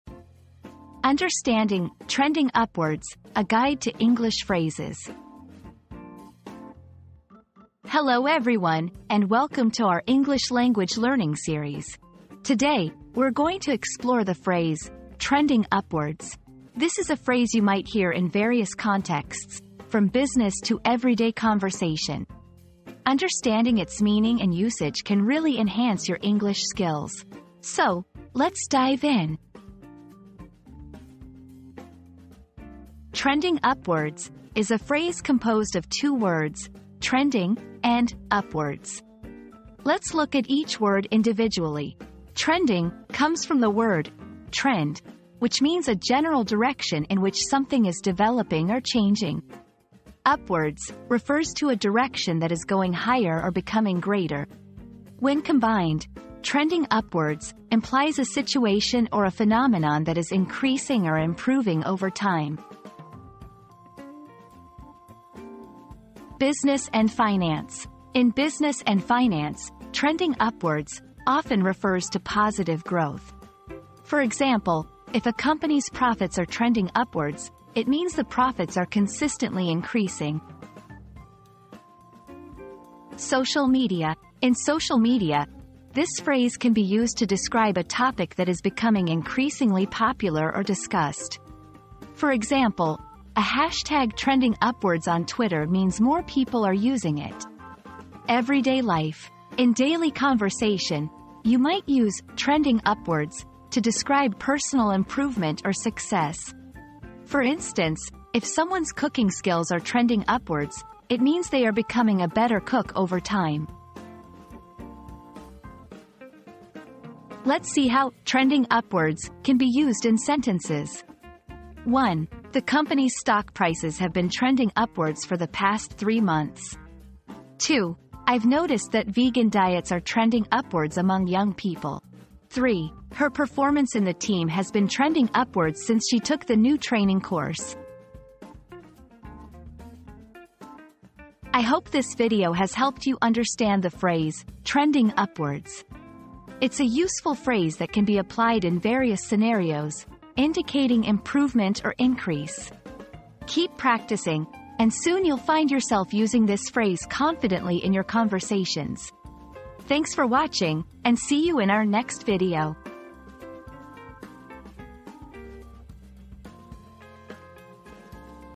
Understanding "Trending Upwards": A Guide to English Phrases
00:00 • Introduction - Understanding "Trending Upwards": A Guide to English Phrases
00:32 • Breaking Down the Phrase
01:05 • Usage in Different Contexts
01:56 • Examples in Sentences